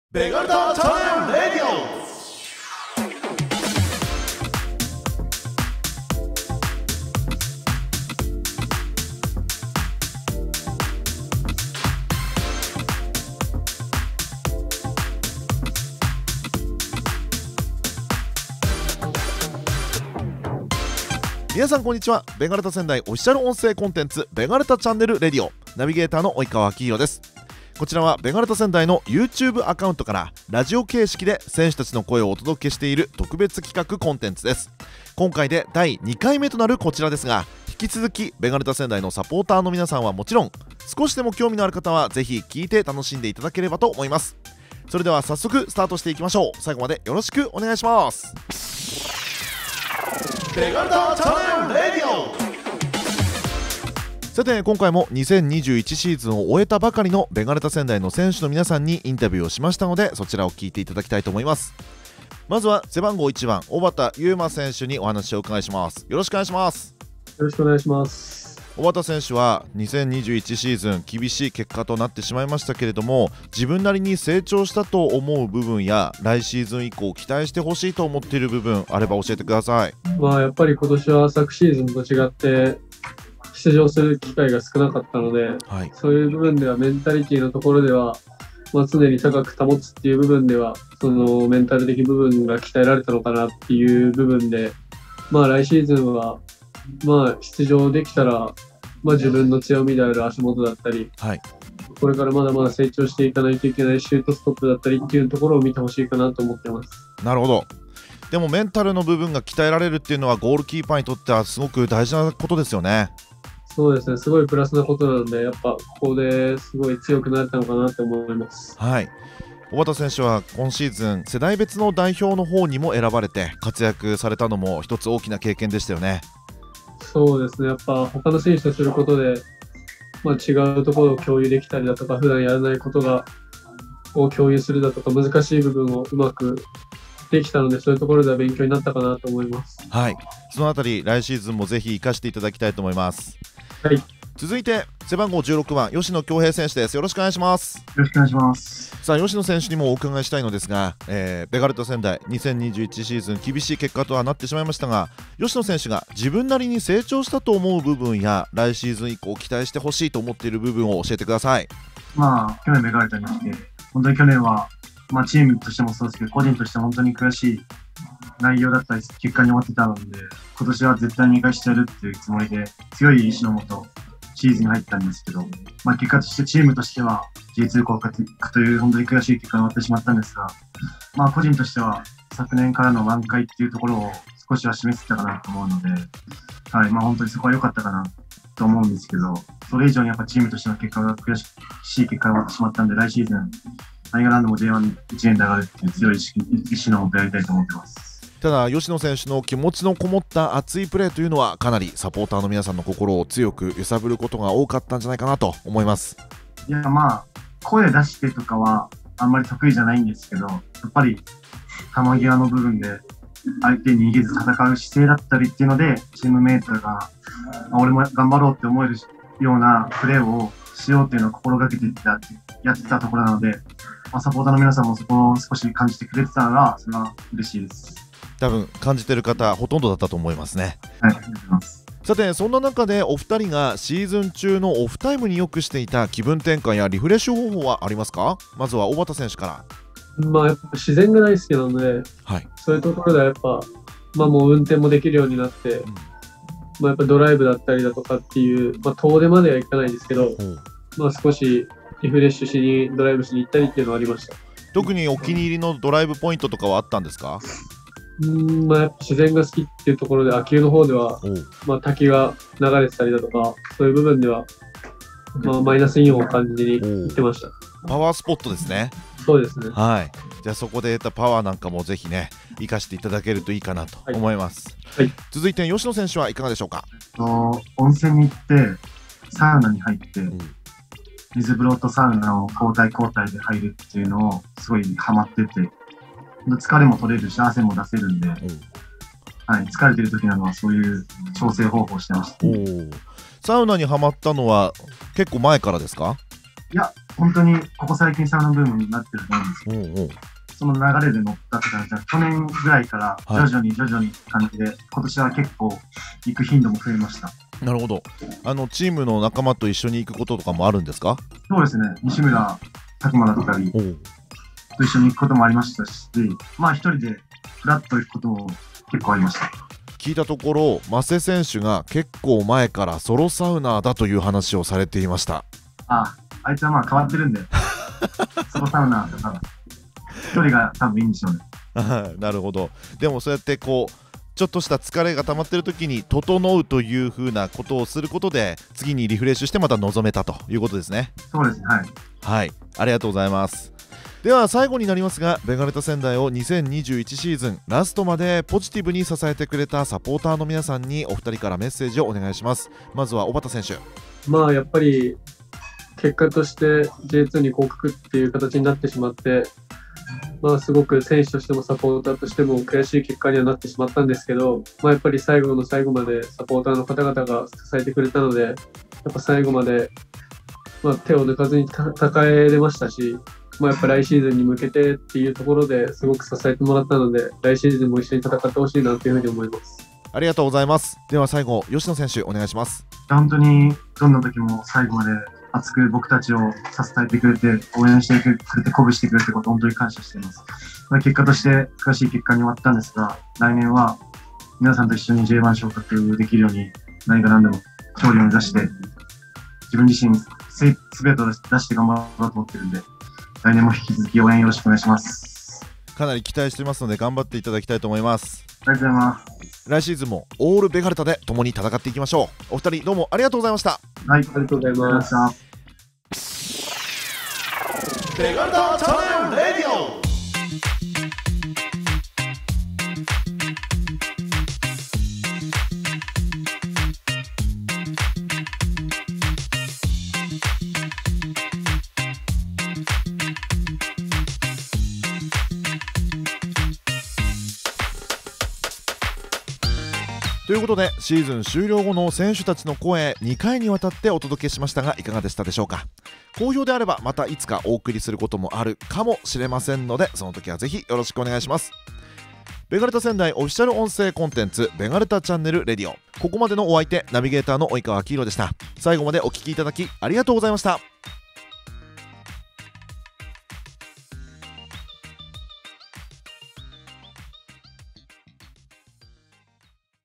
ベガルタチャンネル☆レディオ(ベータ版) vol.02
ベガルタ仙台オフィシャル音声コンテンツ
ナビゲーター：及川アキヒロ
登場選手：GK 1 小畑裕馬、DF 16 吉野恭平

公式YouTubeチャンネルを利用したベガルタ仙台オフィシャル音声コンテンツです。
ナビゲーターの及川アキヒロさんが2021シーズンを終えた選手にインタビュー、GK 1 小畑裕馬選手、DF 16 吉野恭平選手にシーズンの振り返り、オフタイムなどを聞きました。
※このプログラムはベータ版です。2022シーズンの実施に向けたプログラムです。